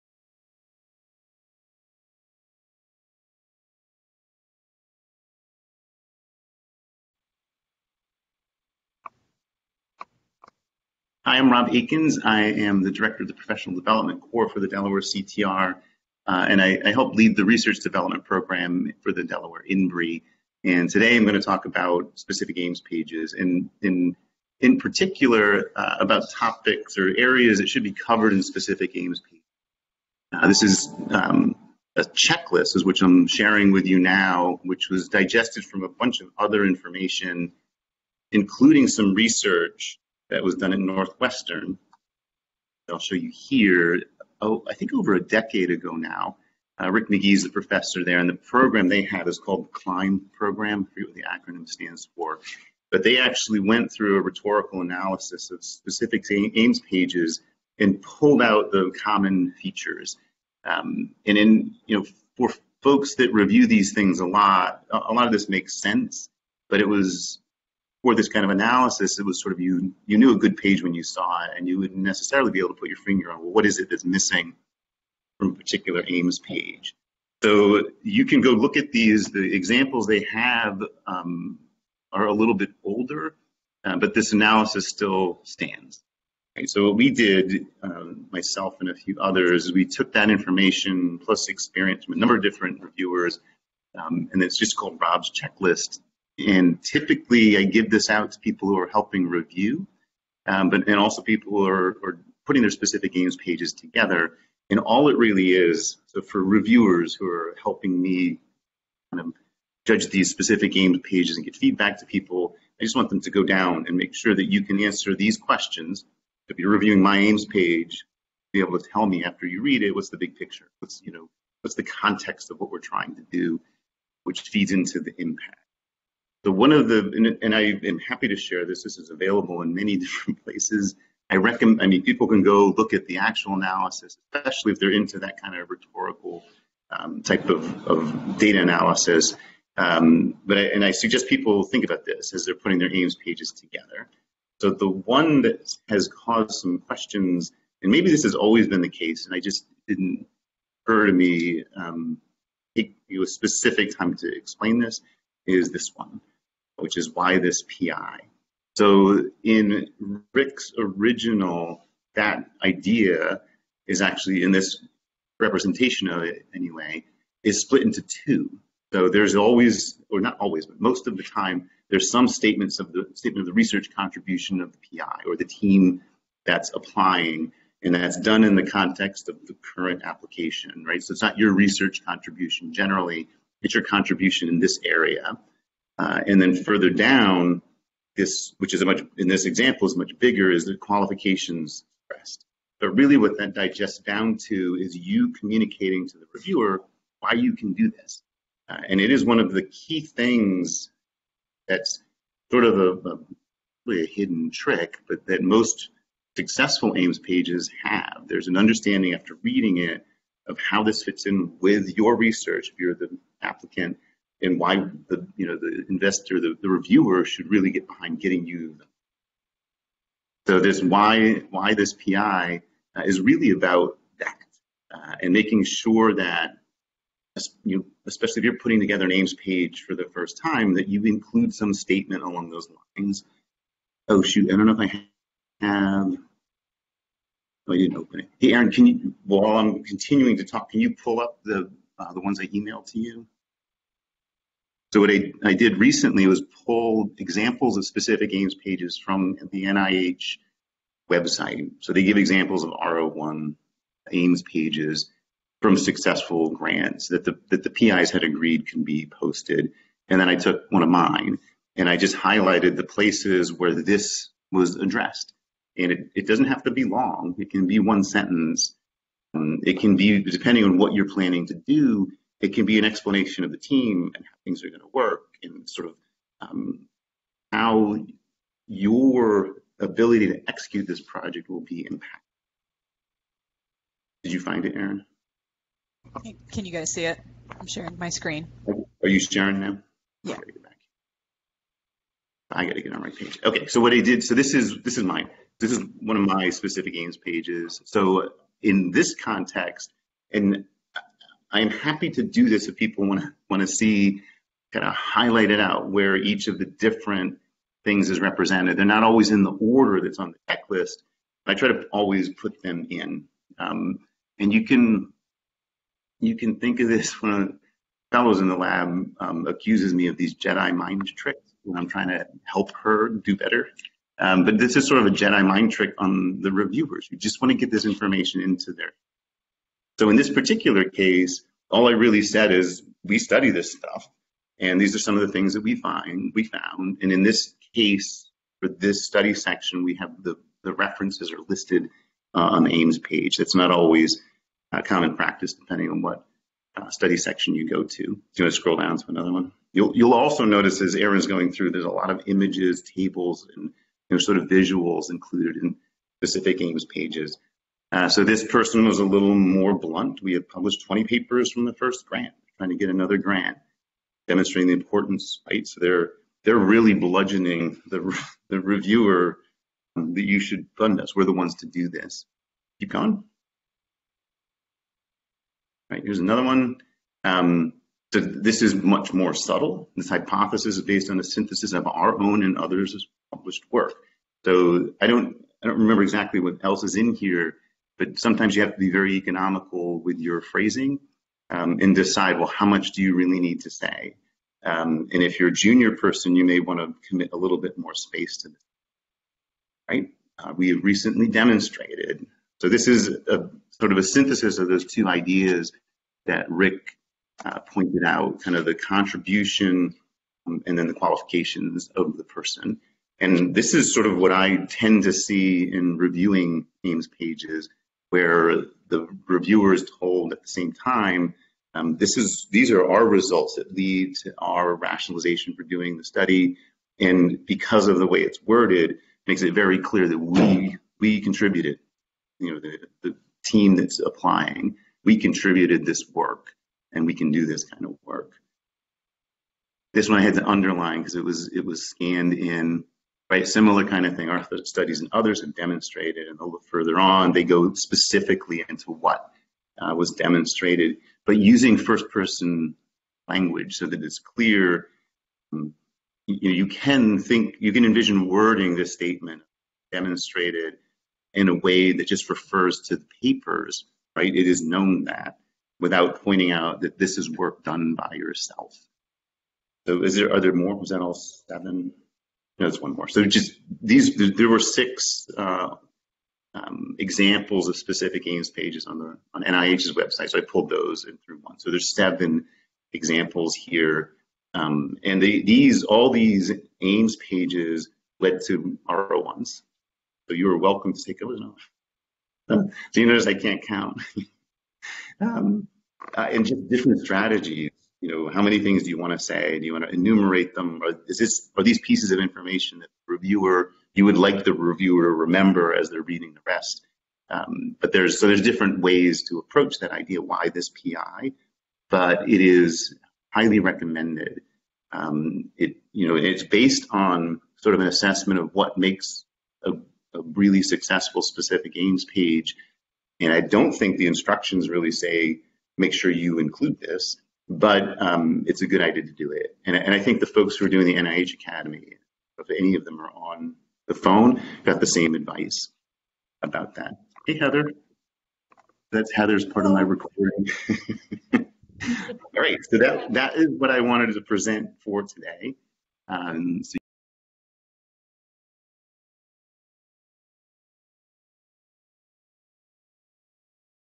Rob's Specific Aims Checklist -- Addressing "Why this PI?"
Presented by Robert Akins, PhD